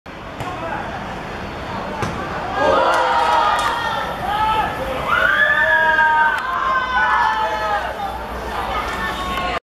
陳呂重德vs信義(2014.5.10.元朗學界足球丙組八強)之入球1:0(絶殺烏龍波)
完塲前一分鐘入球